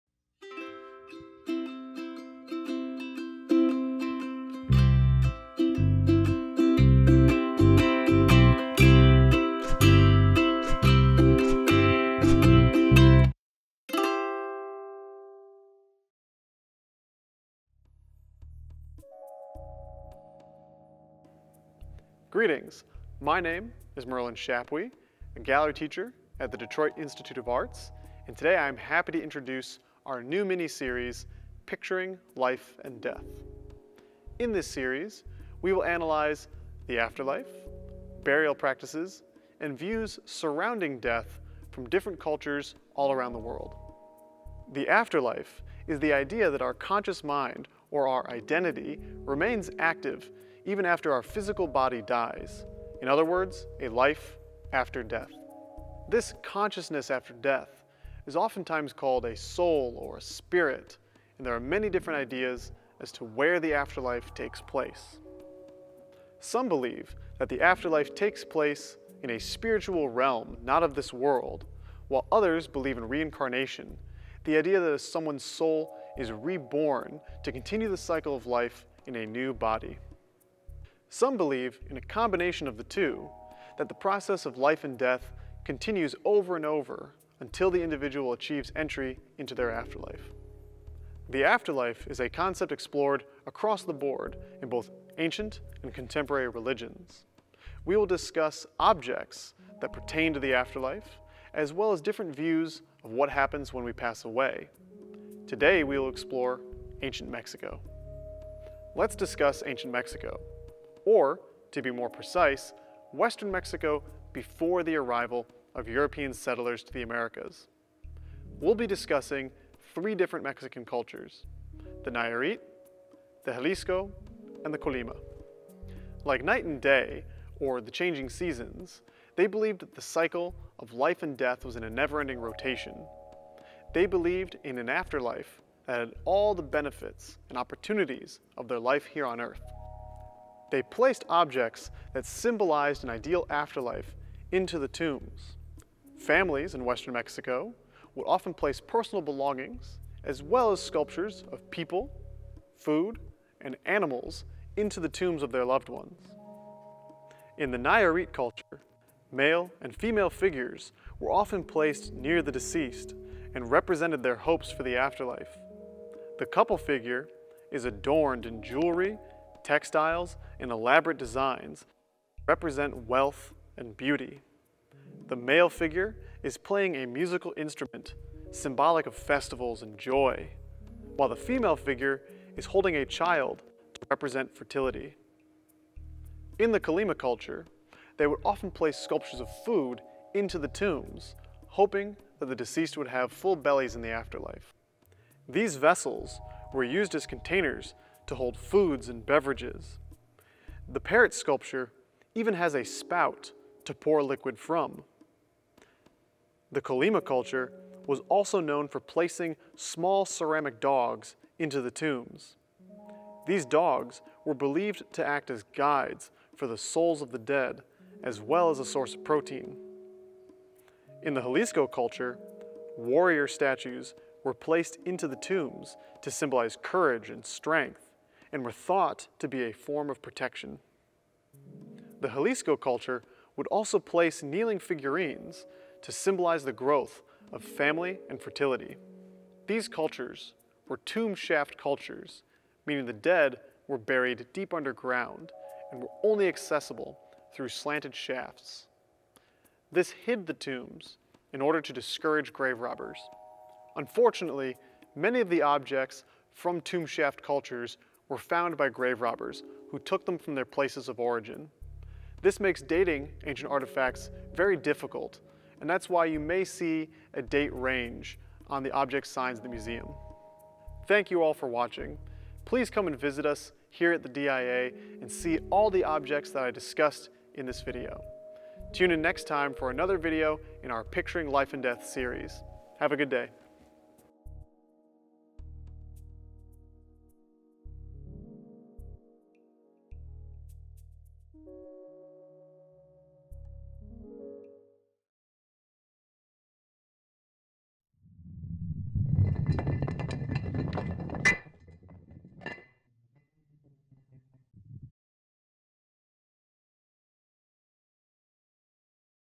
Art Bytes: Picturing Life & Death
In this Art Bytes mini-series, we analyze the afterlife, burial practices, and views surrounding death from different cultures around the world with DIA gallery teacher Merlin Chappuis.